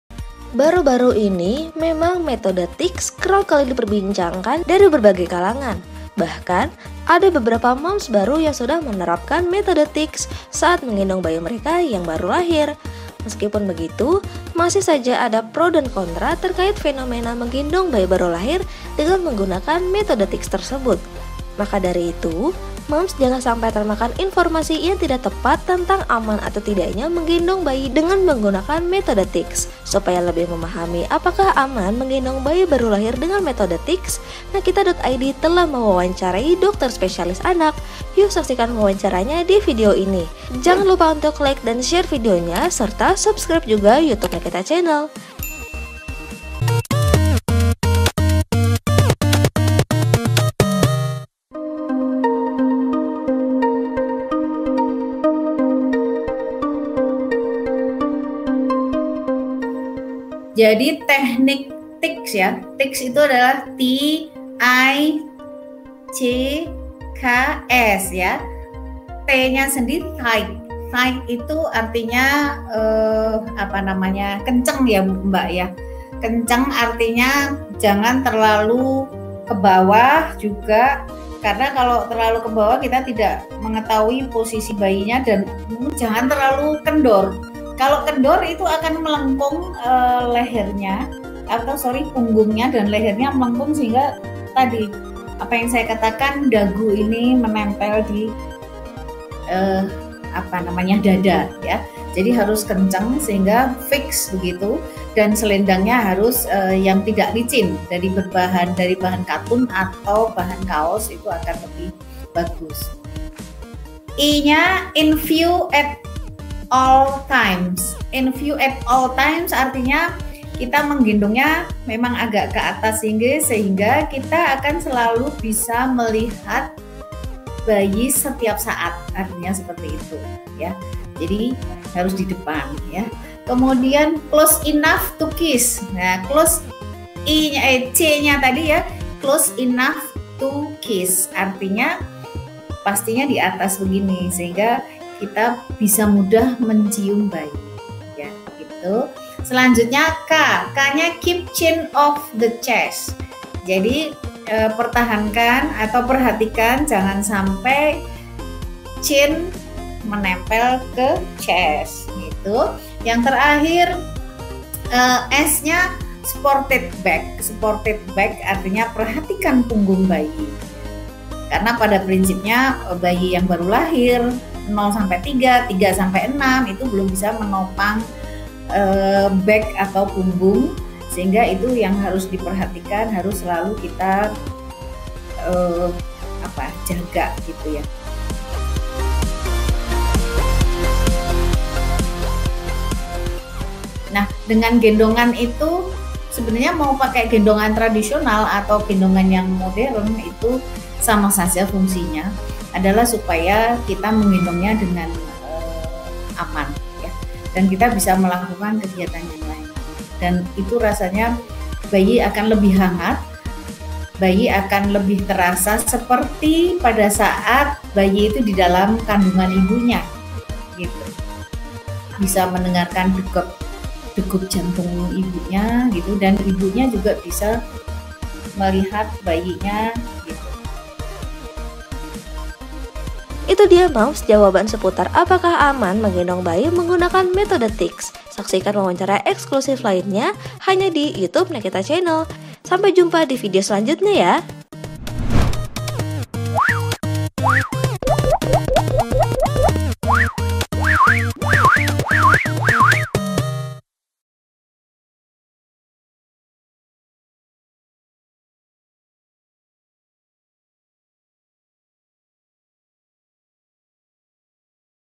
Apakah Aman Menggendong Bayi dengan Metode TICKS?
Baru-baru ini memang metode TICKS kerap kali diperbincangkan dari berbagai kalangan Moms. Bahkan, ada beberapa Moms baru yang sudah menerapkan metode TICKS saat menggendong bayi mereka yang baru saja lahir. Meskipun begitu, masih saja ada pro dan kontra terkait fenomena menggendong bayi baru lahir dengan menggunakan metode TICKS tersebut? Maka dari itu Moms, jangan sampai termakan informasi  tidak tepat tentang aman atau tidaknya menggendong bayi dengan menggunakan metode TICKS.

Supaya lebih memahami apakah aman menggendong bayi baru lahir  dengan metode TICKS, Nakita.id telah mewawancarai dokter spesialis anak.
00:00 - Intro
00:52 - Apakah yang dimaksud dengan metode TICKS?
03:08 - Lebih baik memakai gendongan tradisional atau modern jika sesuai dengan kaidah TICKS?

EDITORIAL OFFICE

ADVERTISING